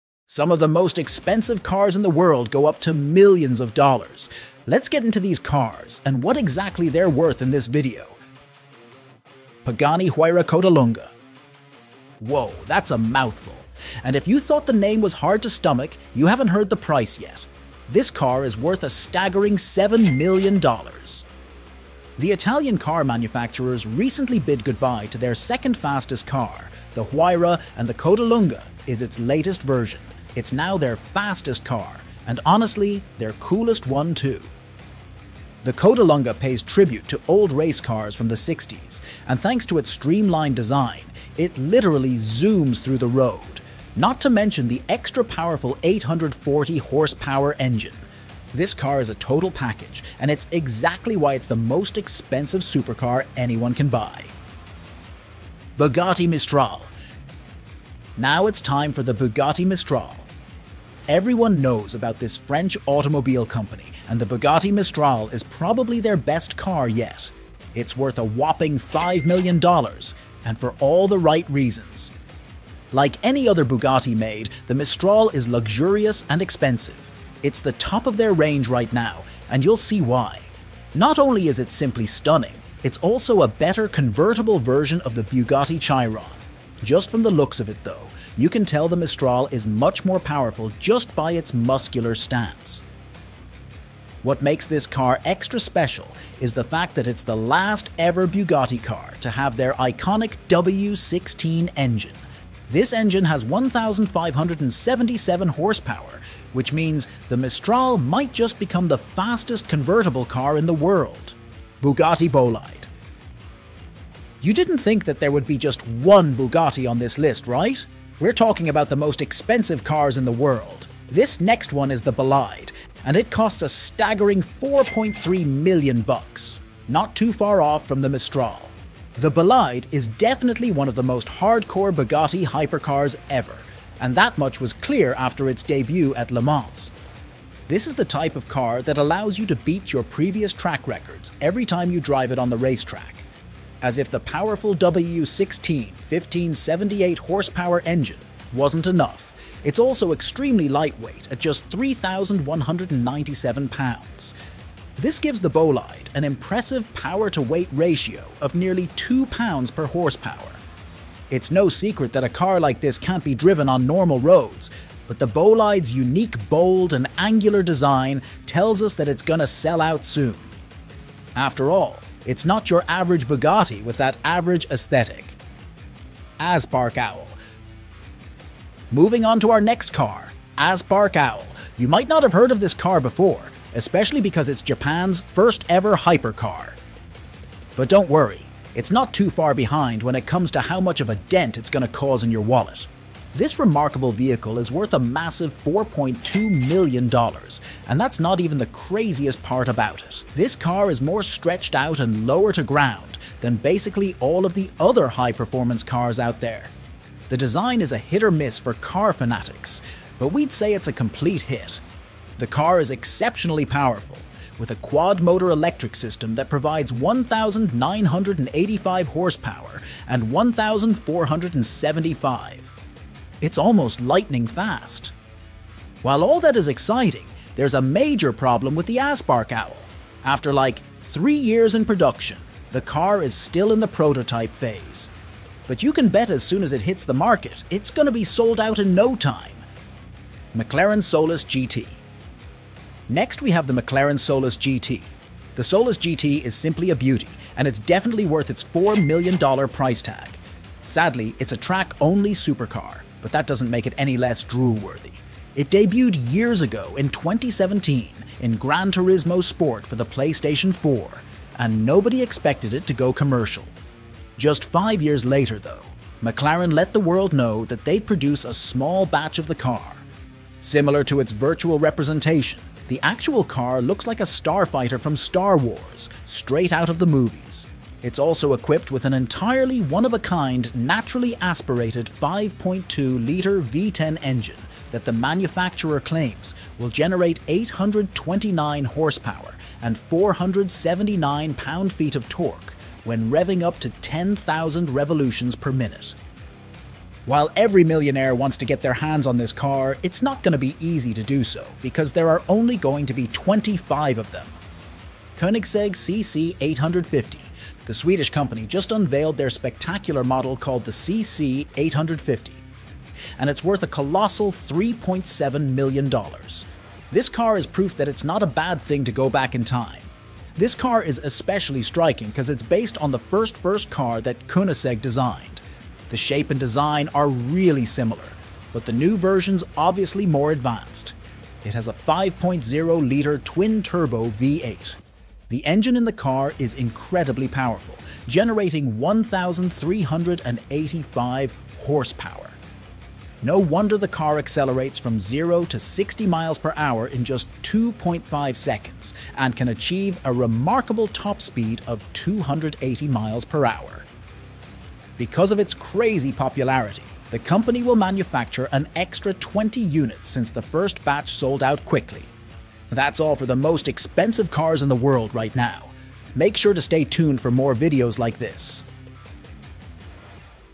The Most Expensive Car in The World (2024)
In this video, we take a look at the most expensive car in the world in 2024. The car is called the 'Limo'. It costs a whopping 1,000,000,000 US dollars!

So who's going to be able to afford to buy this car? It's doubtful that the average person on the street will be able to afford this car, that's for sure. But if you're looking for a piece of luxury that you can actually afford, the Limo might be the car for you.

💬 Share your thoughts on these royal couples in the comments below,